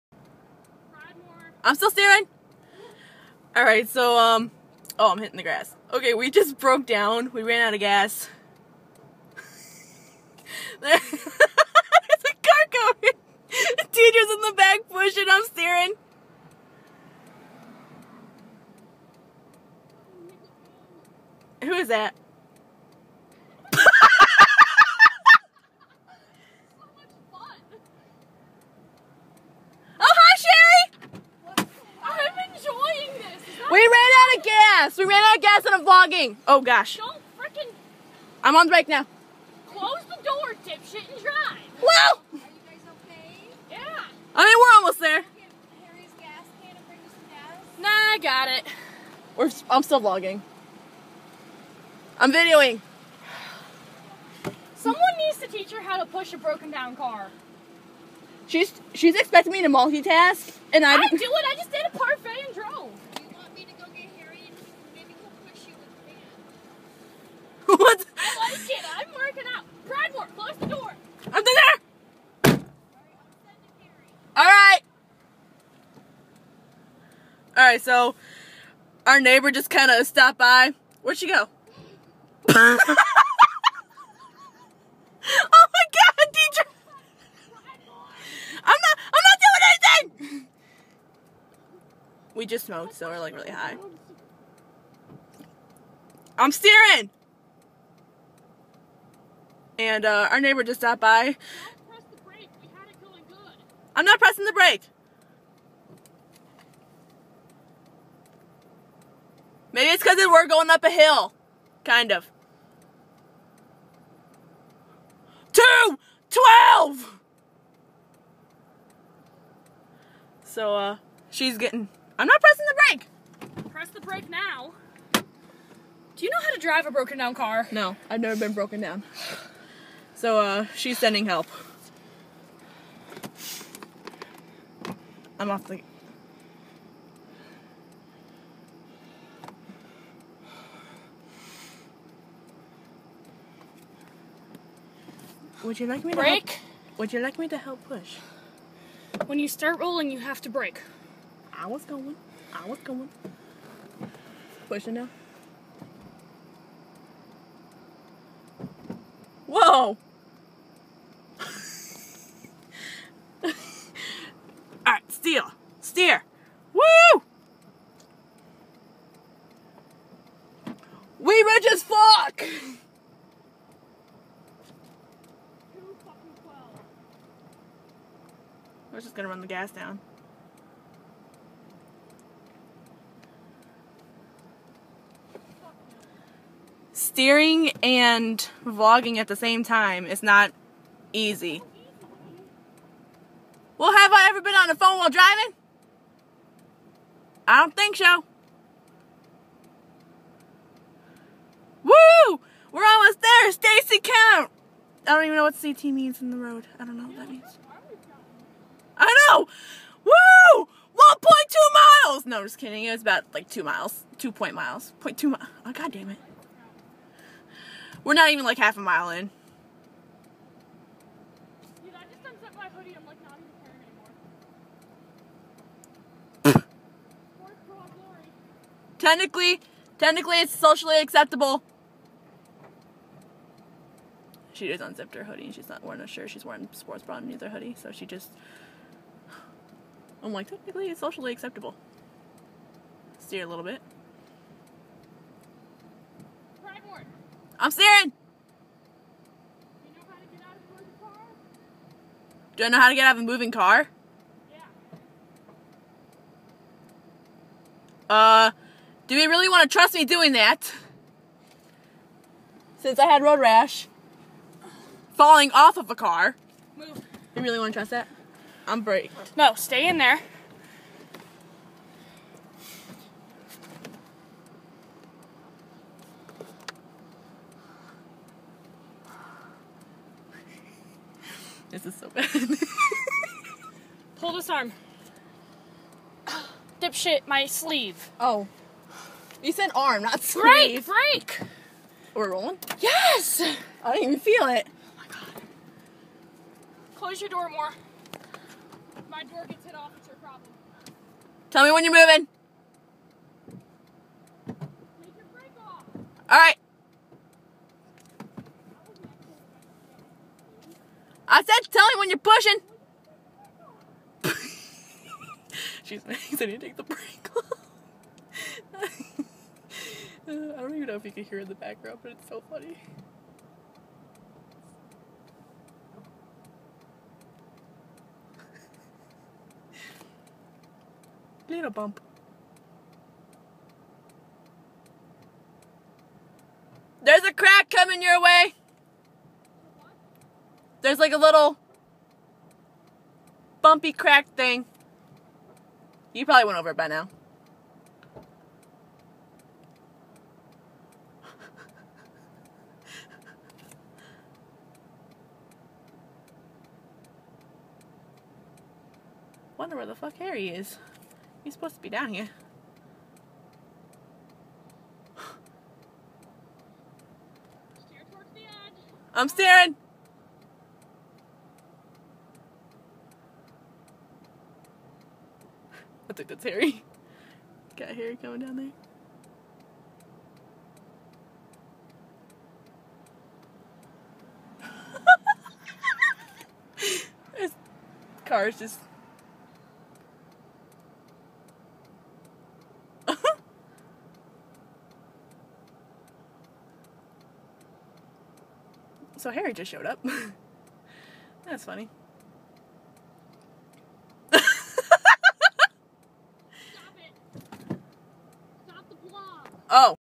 Car Breakdown (Part 1)- The Struggle Begins
So this isn't staged, we didn't plan to run out of gas as we reached the 2 neighborhoods connecting to mine. We never planed for my neighbor to drive by & send help.

It was honestly a hard & tiring situation  most people wouldn't want to be in, but I found it to be a nice workout & pretty fun.